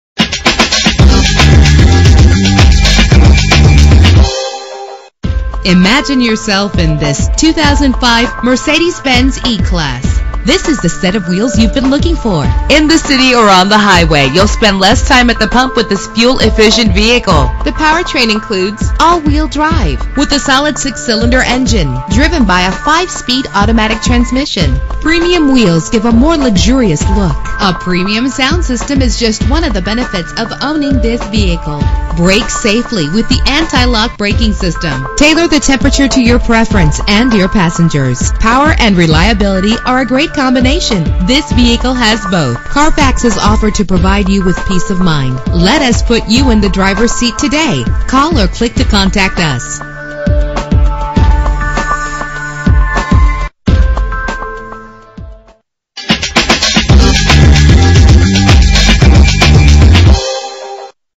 2005 Mercedes-Benz E-Class - MEDINA OH
Year: 2005
Make: Mercedes-Benz
Model: E-Class
Trim: E320 4MATIC
Engine: 3.2 liter 6 cylinder 18 valve
Transmission: 5-Speed Automatic
Color: Capri Blue Metallic
Mileage: 88049
Address:
5180 MONTVILLE DR